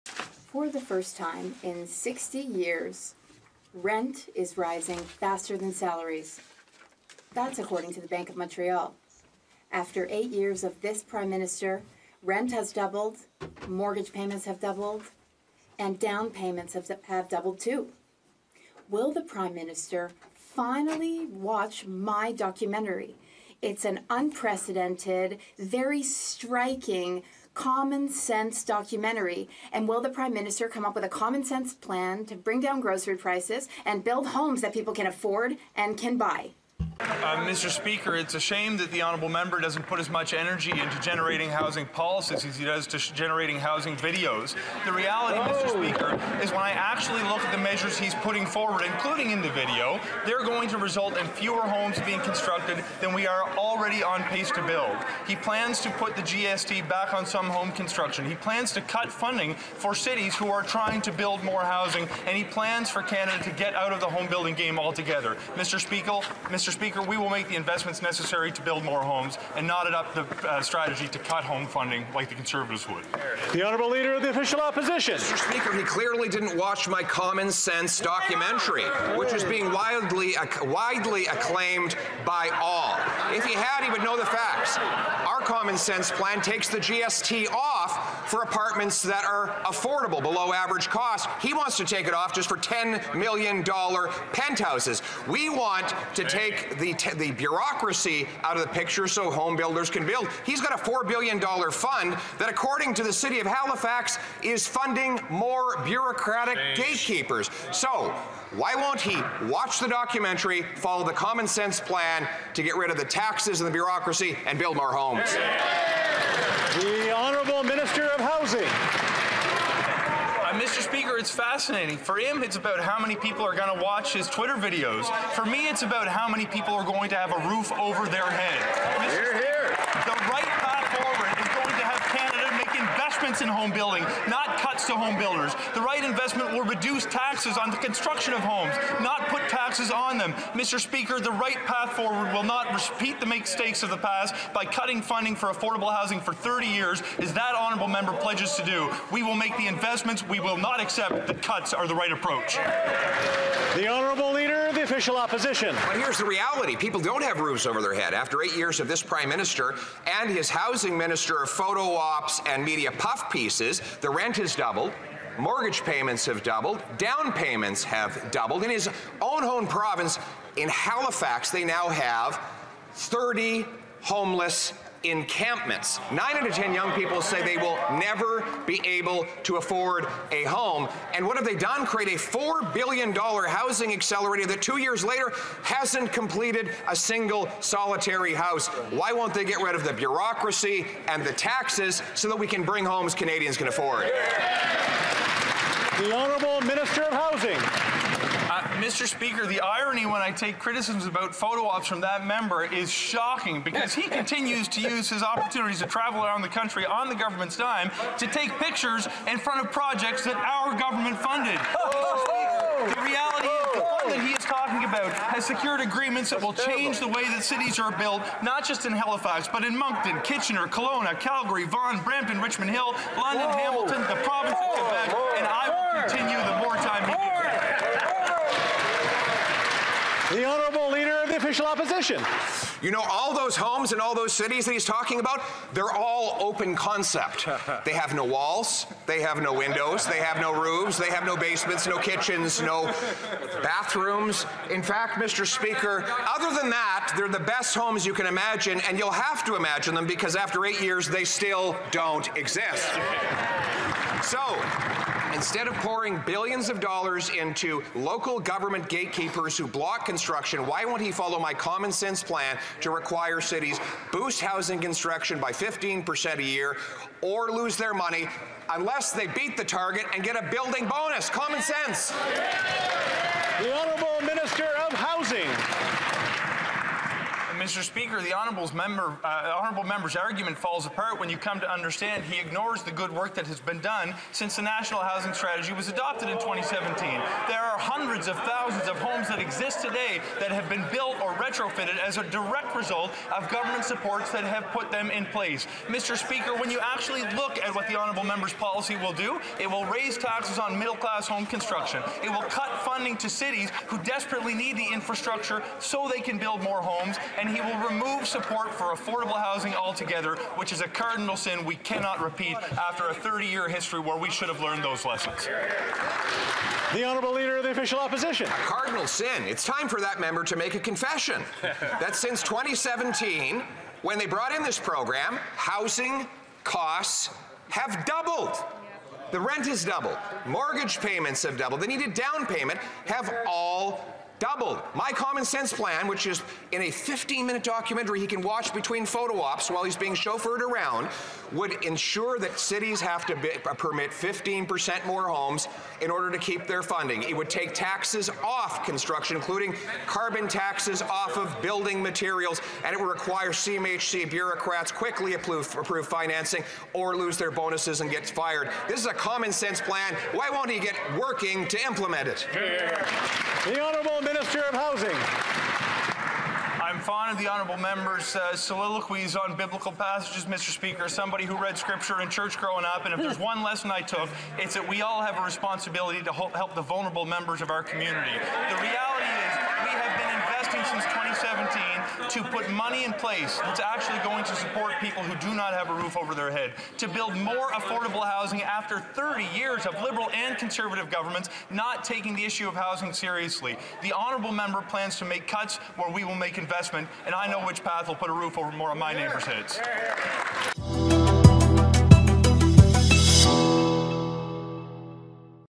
After 8 years, they still don’t exist
'Those Houses Don't Exist'
"After 8 years, they (homes) still don’t exist" ~ Poilievre responds to the Minister of Housing, Infrastructure and Communities of Canada, Sean Fraser, during the House of Commons debate on Canada housing issues.